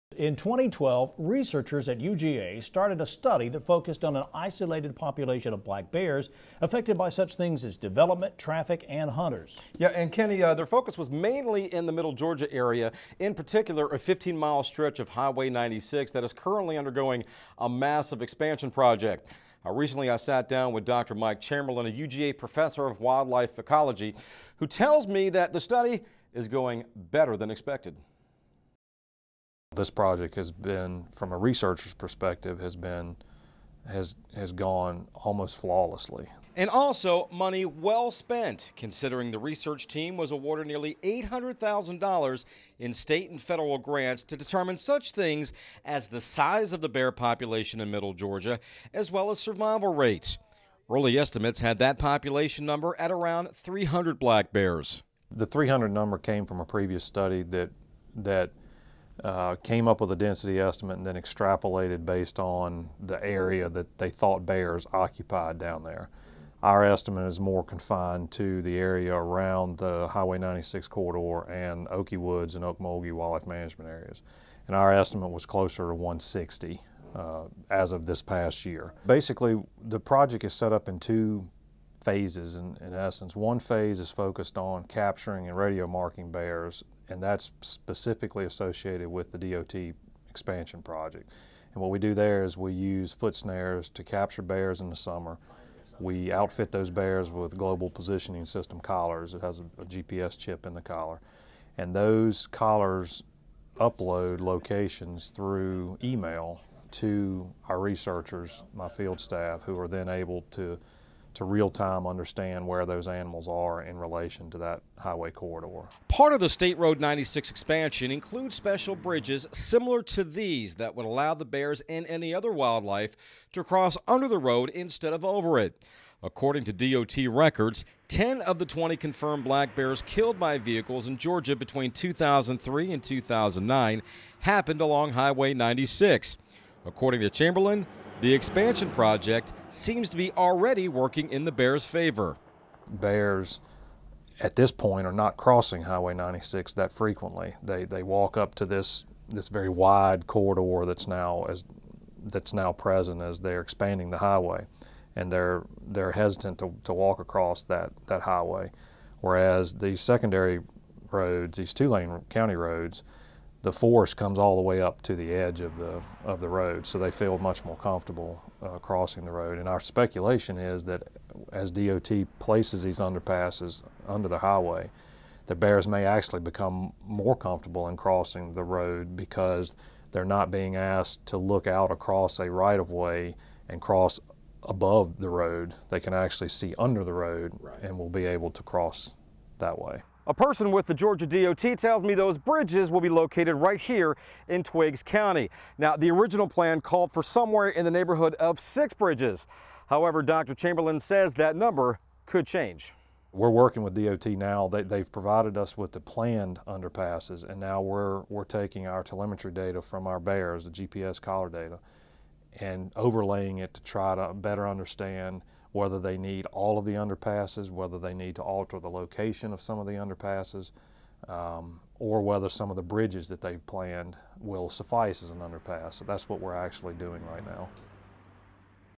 Study Tracks Black Bear Population In Middle Georgia
In 2012, UGA researchers began a study tracking the effects of development, traffic and hunting on the black bear population in one part of Middle Georgia.  The Monitor’s Ray D’Alessio met recently with one of the researchers to see how the study is going.